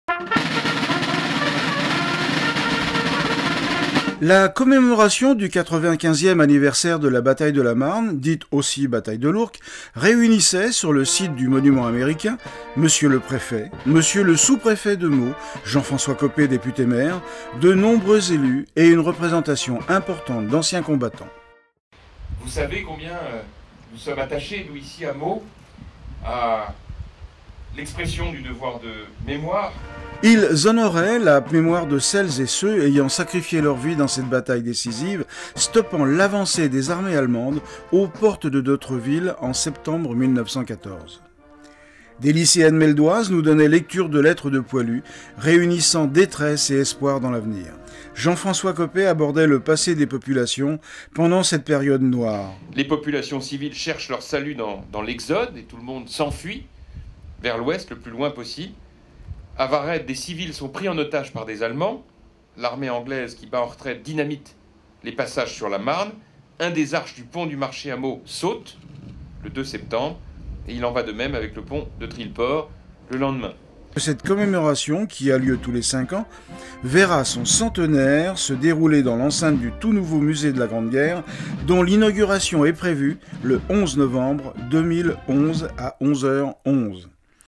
95ème anniversaire de la bataille de la Marne (Meaux)
Les anciens combattants, ainsi que les élus et quelques lycéens ont commémorés le 95ème anniversaire de la bataille de la Marne. (Droits réservés.